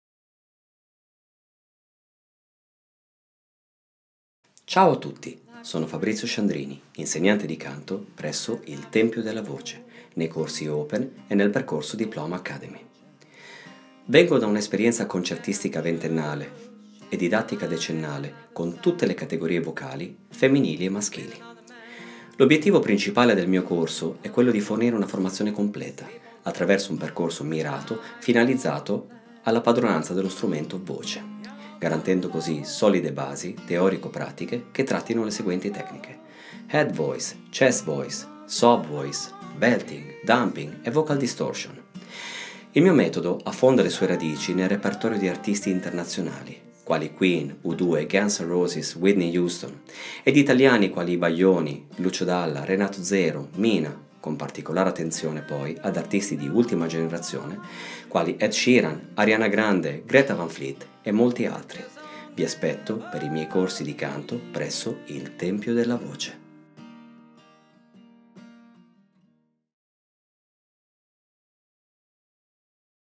Presentazione corsi di Canto | M° Fabrizio Sciandrini
Percorso Diploma Academy & Corsi Open

PERCOSO DIPLOMA ACADEMY Il corso diploma è un percorso accademico professionale triennale, che si pone l’obiettivo di fornire una formazione a 360° del canto e delle materie ad esso complementari. Il programma consente di raggiungere un ottimo livello di indipendenza riguardo la produzione di contenuti, in linea con il mercato musicale attuale, e acquisire un vocabolario tecnico universale che consenta di comunicare con tutte le figure professionali del settore. L’allievo verrà preparato per sostenere gli esami di certificazione internazionale rock & pop presso Trinity College London.

L’ obbiettivo principale del mio corso è quello di fornire una formazione completa attraverso un percorso mirato, finalizzato alla padronanza dello strumento voce, garantendo solide basi teorico/pratiche che trattino le seguenti tecniche:

Head voice
Chest voice
Sob voice
Belting
Dumping
Vocal distortion

Il mio metodo affonda le sue radici nel repertorio di artisti internazionali quali:
Queen, U2, Gun’s & roses, Whitney Huston

Ed italiani quali:
Baglioni, Lucio Dalla, Renato Zero, Mina,

con particolare attenzione ad Artisti di ultima generazione quali:
Ed Sheeran, Ariana Grande, Greta van fleet ed altri.

Vi aspetto per i miei corsi di canto presso Il tempio della voce